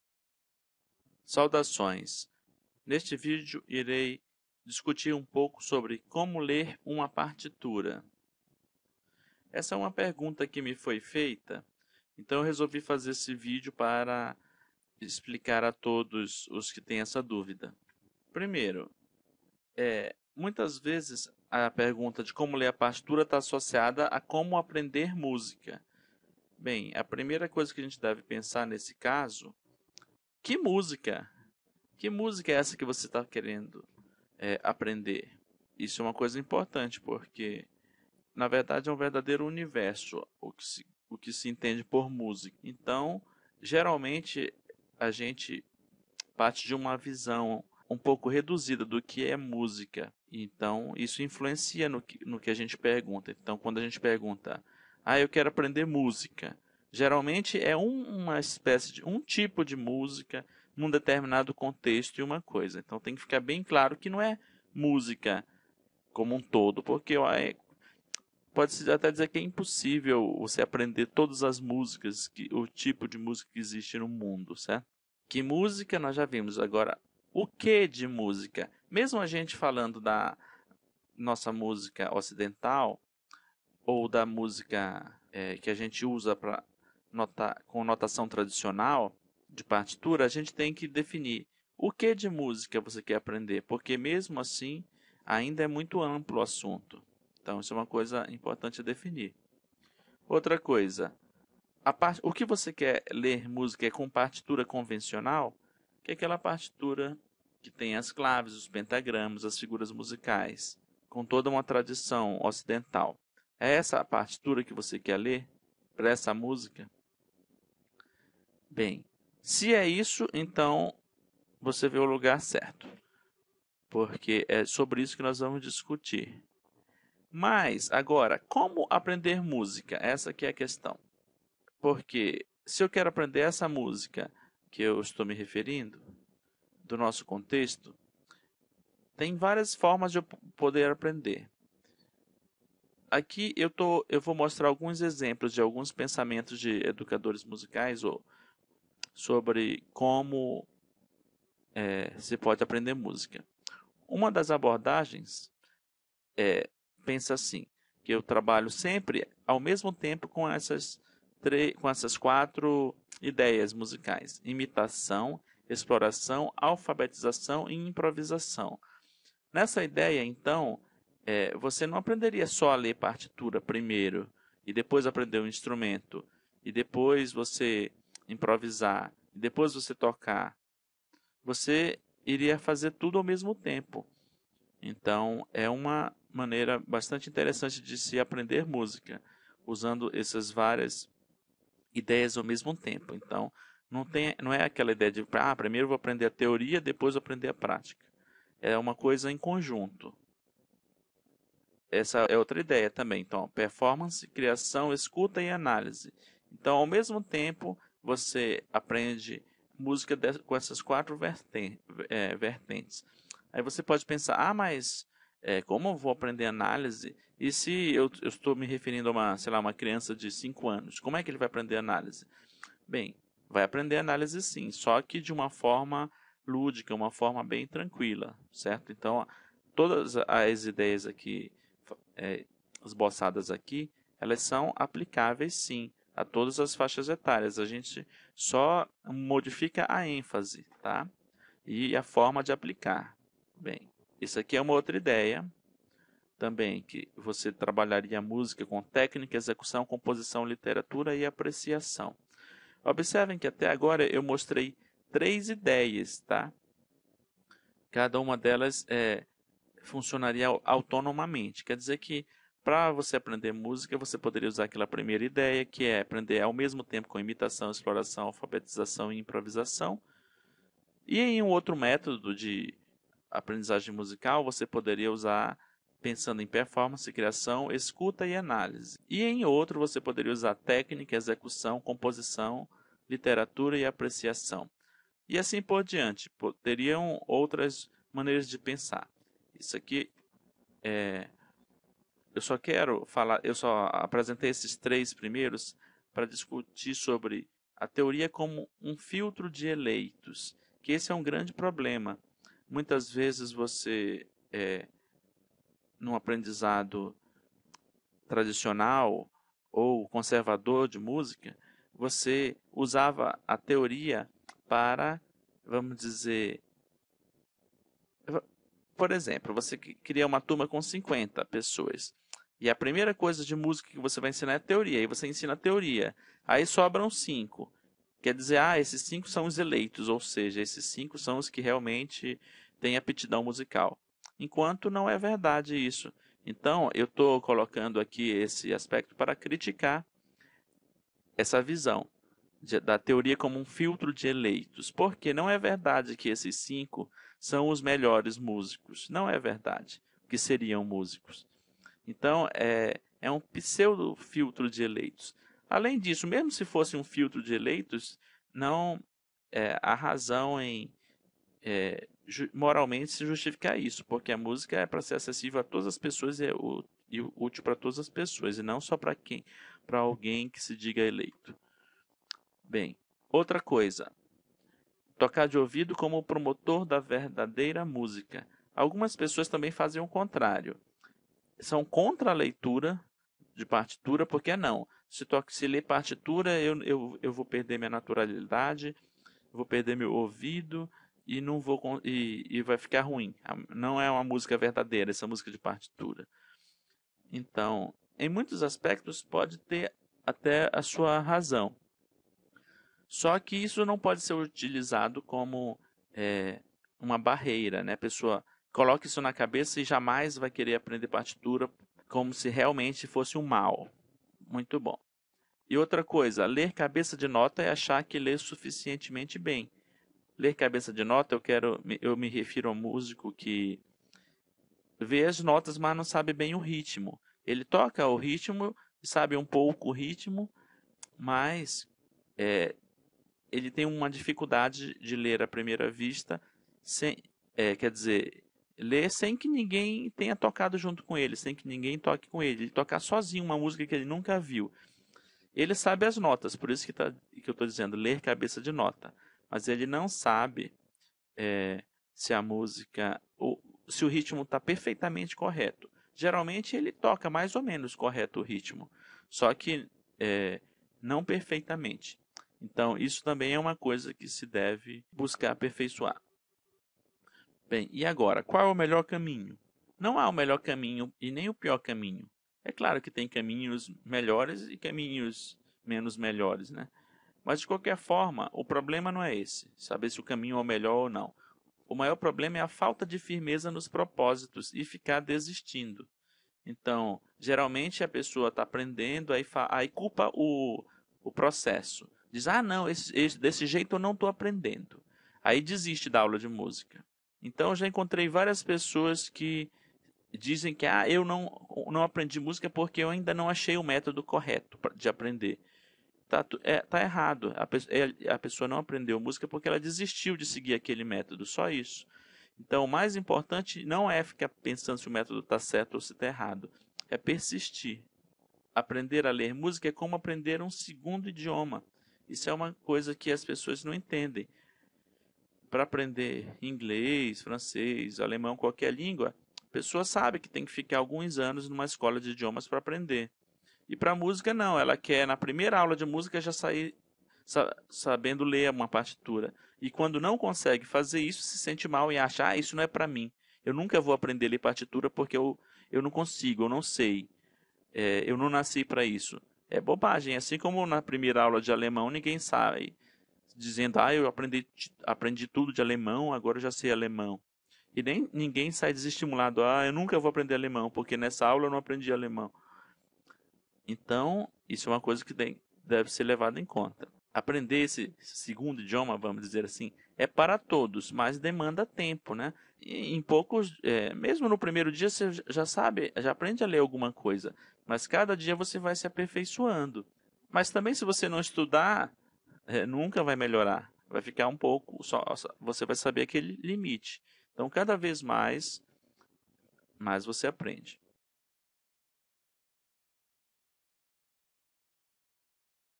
Explanação sobre a Leitura de Partitura - Parte 1
Fala do prof. Glauber Santiago introduzindo o estudo à leitura de partitura

Explicação sobre a Leitura de Partitura - Parte 1

00:01 Como Ler uma Partida
Visão geral da seção: neste vídeo, o palestrante discute como ler uma partitura. O primeiro passo é definir que tipo de música você quer aprender e que tipo de partitura quer ler.

Definindo que tipo de música você quer aprender
É importante definir que tipo de música você quer aprender porque existem muitos tipos diferentes de música.
Mesmo dentro da música ocidental, existem muitos estilos e gêneros diferentes.
Você precisa ser específico sobre o tipo de música que deseja aprender antes de começar a aprender a ler a partitura.
Diferentes Abordagens para Aprender Música
Existem várias abordagens para aprender música, incluindo imitação, exploração, alfabetização, improvisação, performance, criação, audição, análise, técnica, execução, composição, literatura e apreciação.
Alguns educadores sugerem usar todas as quatro ideias ao mesmo tempo: imitação, exploração, alfabetização e improvisação.
Outros educadores sugerem focar na performance, criação, escuta e análise ou técnica de execução, composição, literatura e apreciação.
Essas ideias podem ser aplicadas em todas as faixas etárias.
Criticando os Métodos Tradicionais de Educação Musical
Os métodos tradicionais costumam usar a teoria como um filtro para selecionar alunos com aptidão musical.
Essa abordagem é falha porque pressupõe que apenas alguns poucos têm talento musical quando, na realidade, qualquer um pode aprender música.
Além disso, mesmo que fosse verdade que apenas alguns poucos tinham talento musical, não há justificativa moral para excluir outros do aprendizado.
06:35 Aprender a ler música
Visão Geral da Seção: Nesta seção, o palestrante discute como aprender a ler música e enfatiza a importância da persistência no processo de aprendizagem.

A melhor maneira de aprender música
Aprender música é como aprender uma segunda língua.
Leva tempo e persistência para aprender a ler música.
Não existe a melhor ou a pior maneira de aprender música; o que mais importa é ficar com ela.
Muitas pessoas culpam o processo de aprendizado quando lutam com a leitura de música, mas o problema real costuma ser a falta de comprometimento.
Lendo partituras
Algumas pessoas veem a partitura como uma barreira e se recusam a aprendê-la.
Outros podem ler notas, mas lutam com o ritmo quando tocam sozinhos.
A leitura de partituras exige prática e dedicação, como qualquer outra habilidade.
A persistência é a chave
Aprender a ler música exige tempo e esforço, mas qualquer um pode fazê-lo se for persistente.
As pessoas que desistem de aprender um instrumento muitas vezes culpam seu fracasso por não encontrar o método certo, mas raramente é o caso.

Sumário:
Introdução ao Estudo da Leitura de Partitura
Prof. Glauber Santiago

I-Como Ler uma Partitura (00:01)
A. Visão geral da seção:

Discussão sobre como ler uma partitura.
Importância de definir o tipo de música e partitura desejadas.
A importância de especificar o gênero musical desejado.
Variedade de estilos e gêneros na música ocidental.
Abordagens incluem imitação, exploração, alfabetização, improvisação, entre outros.
Sugestões de educadores sobre métodos de aprendizagem.
D. Criticando os Métodos Tradicionais de Educação Musical:
Problemas com o uso da teoria como filtro na educação musical.
Argumento contra a exclusão no aprendizado musical.

II- Aprender a Ler Música (06:35)

Discussão sobre o processo de aprender a ler música.
Ênfase na persistência durante o aprendizado.
Comparação do aprendizado de música com o aprendizado de uma segunda língua.
A importância da persistência e comprometimento.
C. Lendo Partituras:
Desafios comuns na leitura de partituras.
A prática e dedicação necessárias para ler partituras.
D. A Persistência é a Chave:
Encorajamento para a persistência no aprendizado de música.
Enfatizando que qualquer um pode aprender se mantiver o esforço.